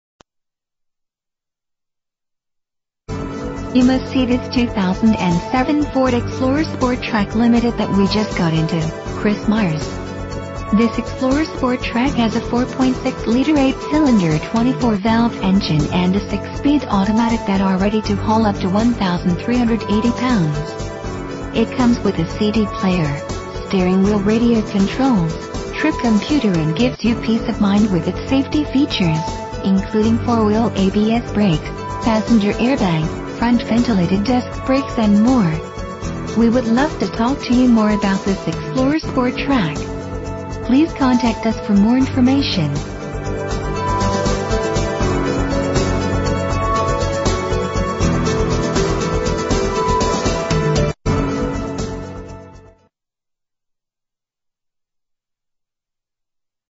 2007 Ford Explorer Sport Trac - Daphne AL
Year: 2007
Make: Ford
Model: Explorer Sport Trac
Engine: 4.6 liter 8 cylinder 24 valve
Transmission: 6-Speed Automatic
Exterior: Silver Birch Clearcoat Metallic
Miles: 44501

Chris Myers
27161 Hwy. 98
Daphne, AL 36526